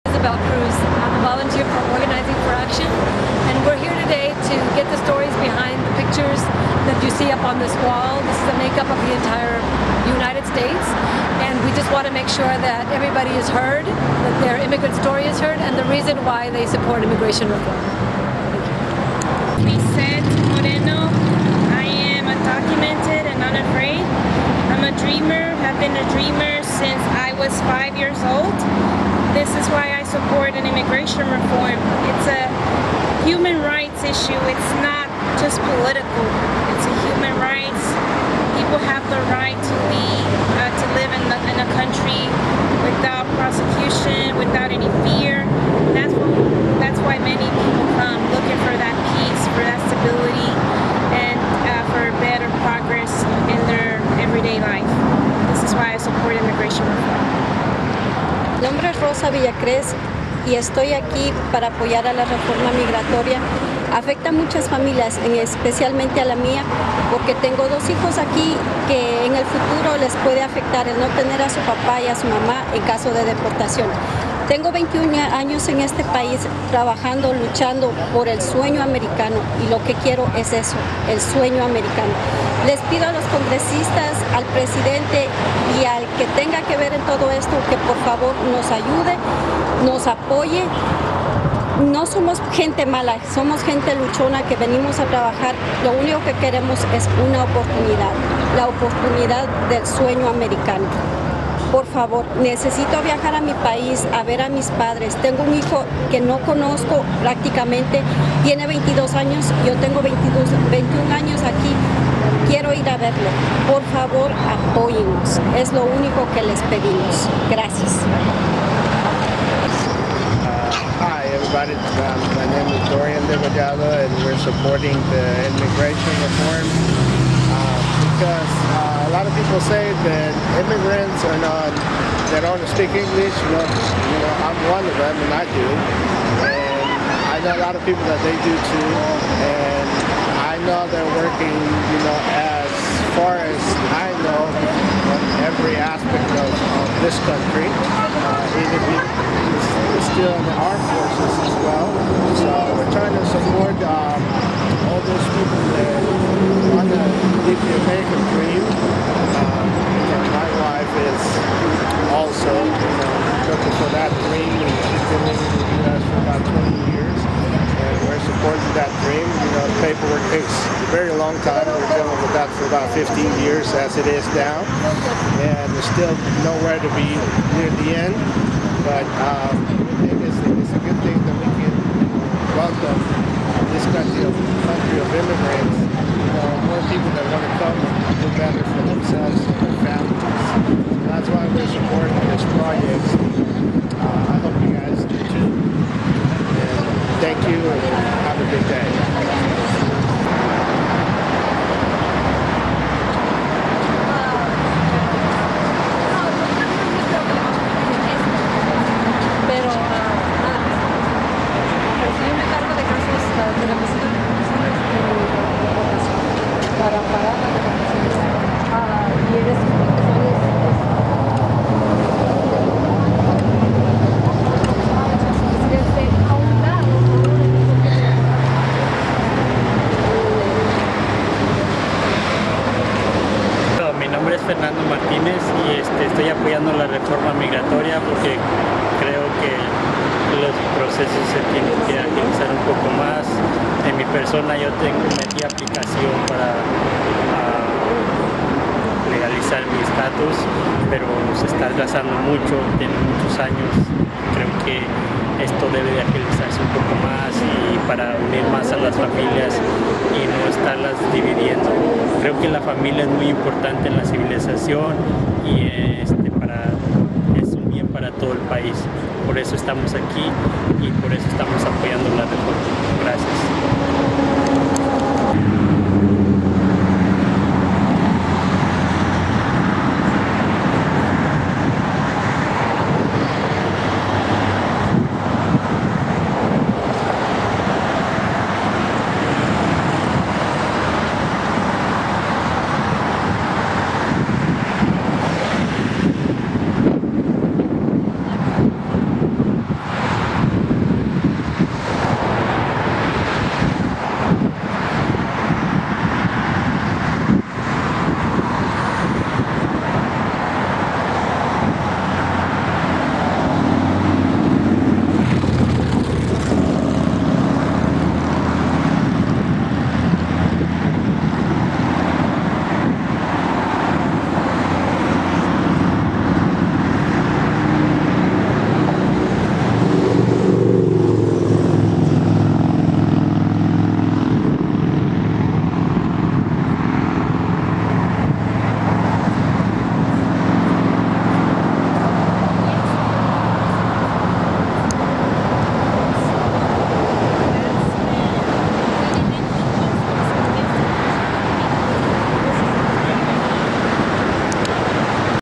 Sep 15 2013 Immigrants for Reform, InsideOut11M at St Vincent de Paul Thrift Store in Dallas
INSIDEOUT11M is a nationwide participatory art initiative aimed at creating a portrait of America that includes immigrants and the descendants of immigrants alike. Immigration Reform, stories behind the people on wall at St Vincent de Paul Thrift Store in Dallas, TX.